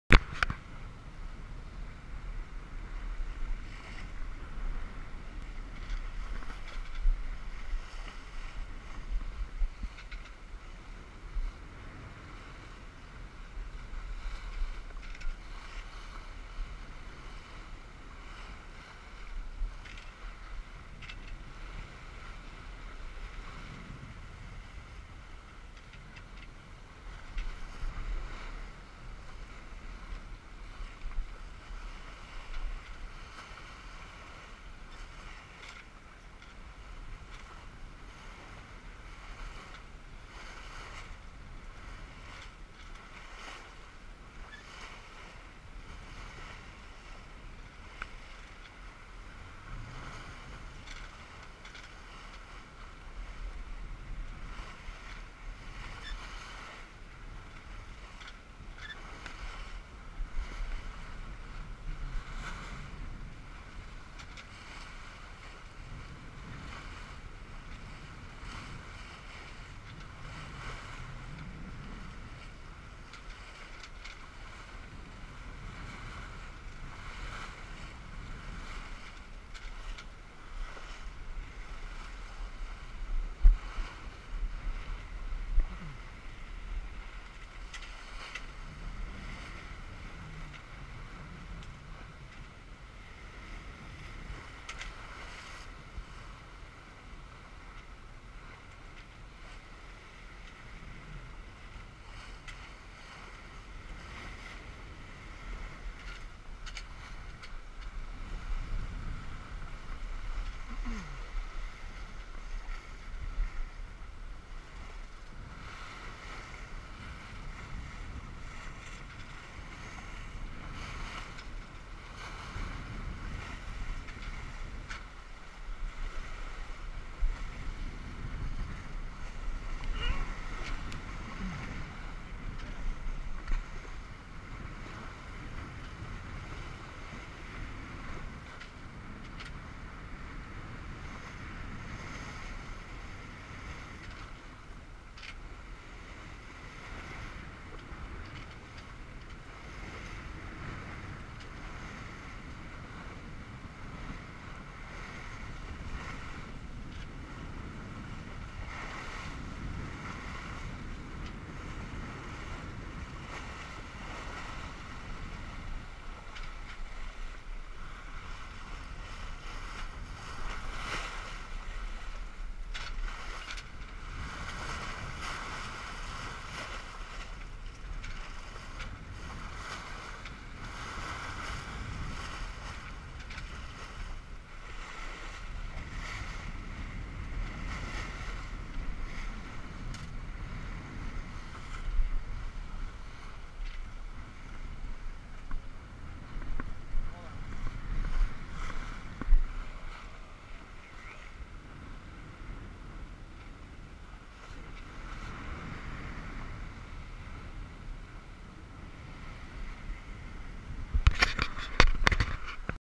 Tanzer up on the deck
Nice beam reach coming from Killarney. experimenting with the headband for the gopro and trying not to tumble into the water!